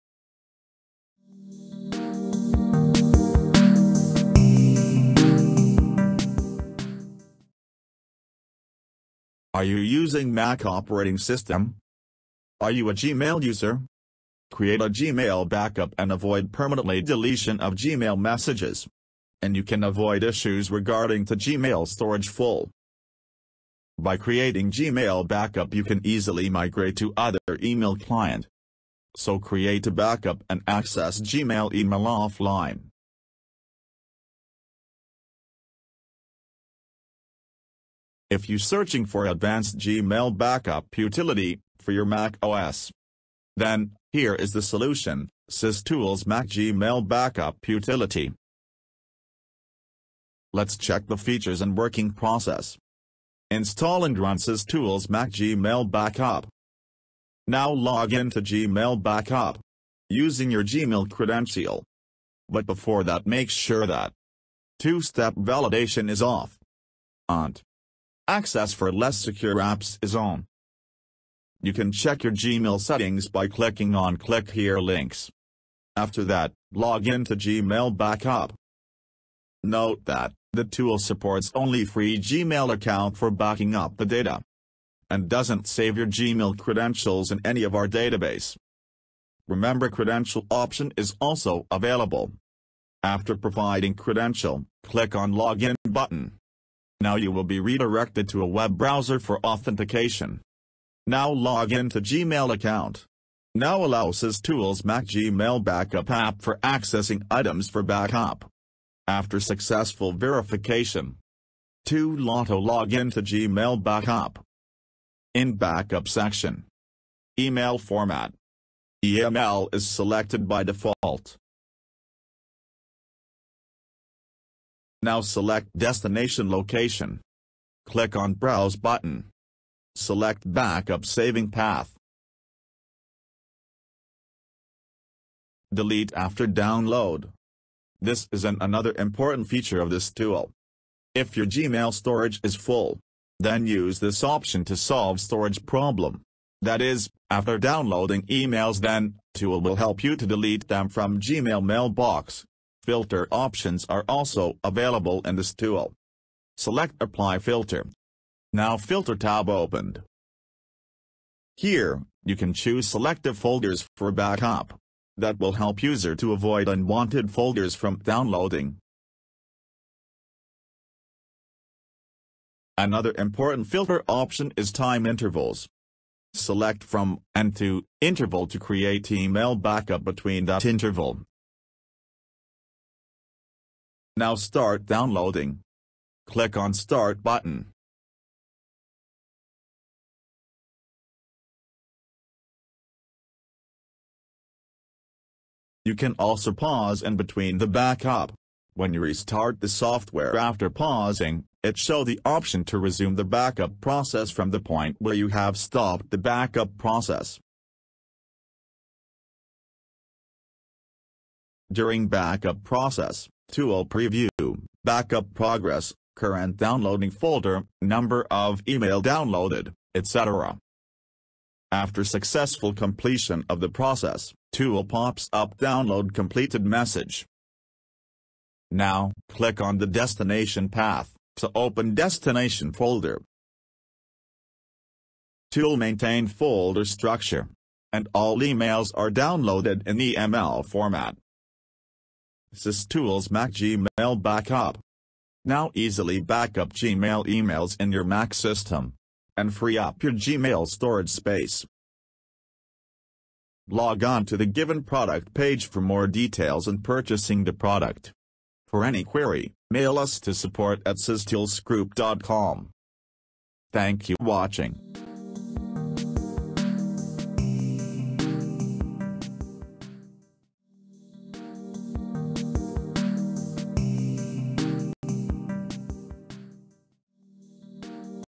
How to take Gmail Backup on Mac OS
Learn how to download Gmail backup for mac. Mac Gmail Backup Software for creating a complete backup of Emails of a Gmail account data on Mac OS machine. Download Gmail emails to Mac to Backup Gmail on Mac.